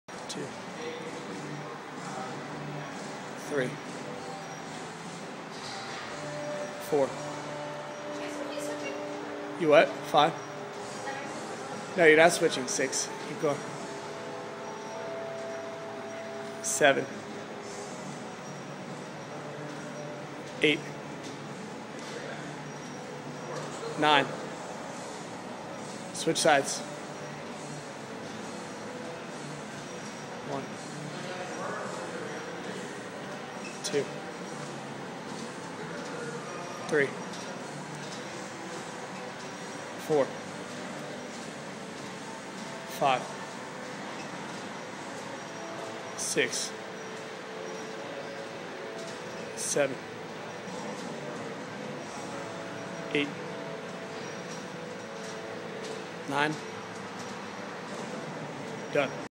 Plank with Knee Raise to Torso Rotation
This exercise targets you core & shoulders.